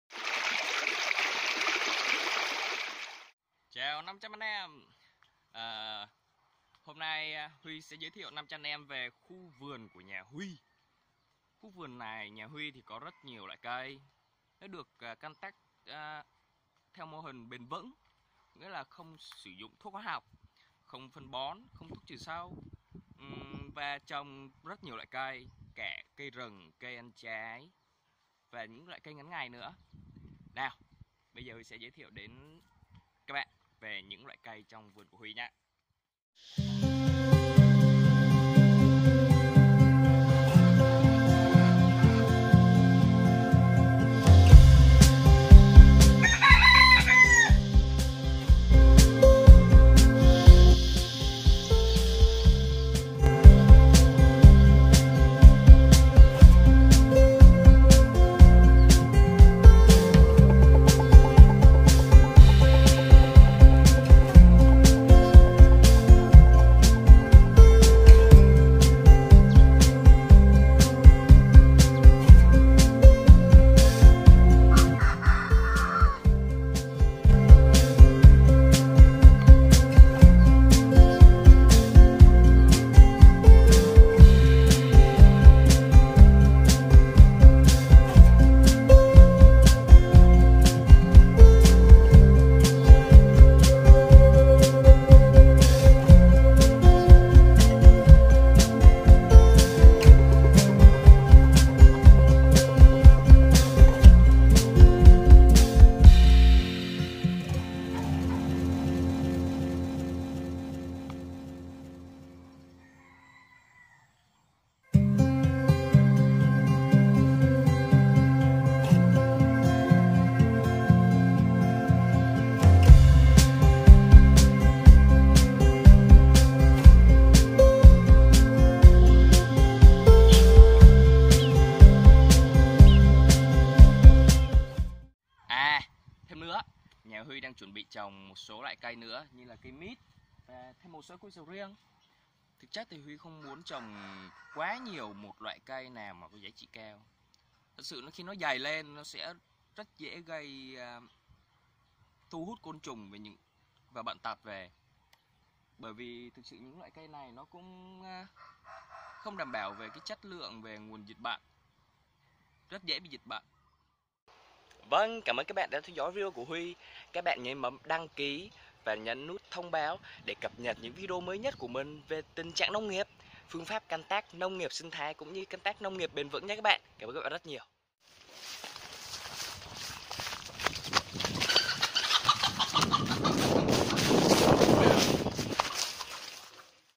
Vườn Trái Cây Sinh Thái Đa Dạng Nhà HUY - Permaculture Việt Nam
Trong đoạn giới thiệu có một sô từ hiểu sai như không sử dụng phân bón có nghĩa là không sử dụng phân bón hóa học